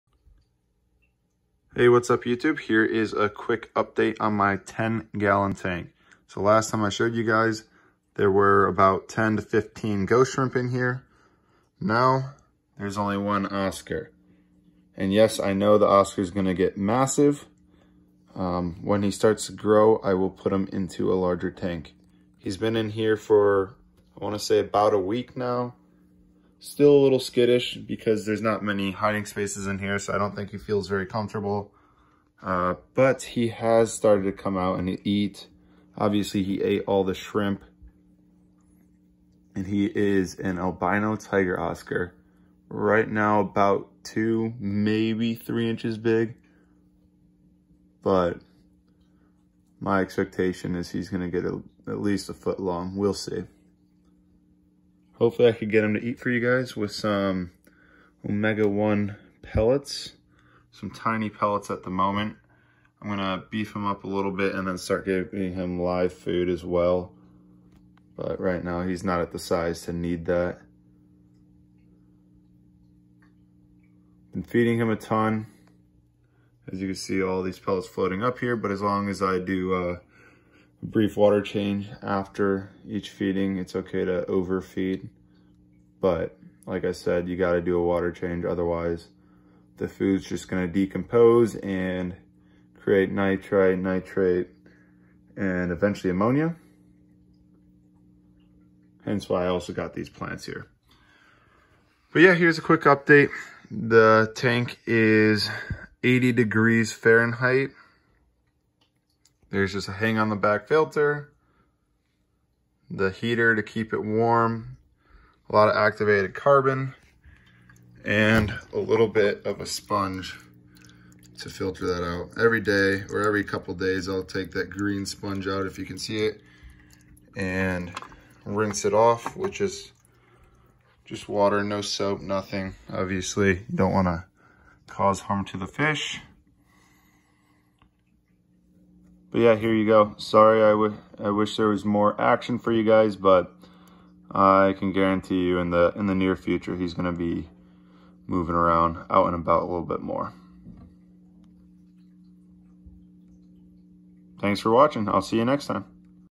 Oscar in a 10 Gallon Tank!?
Got a new oscar and while he is still small I am keeping him in my 10 gallon tank. Don't worry... as he gets bigger I have a 36g, 60g and eventually a 125g tank to put him into!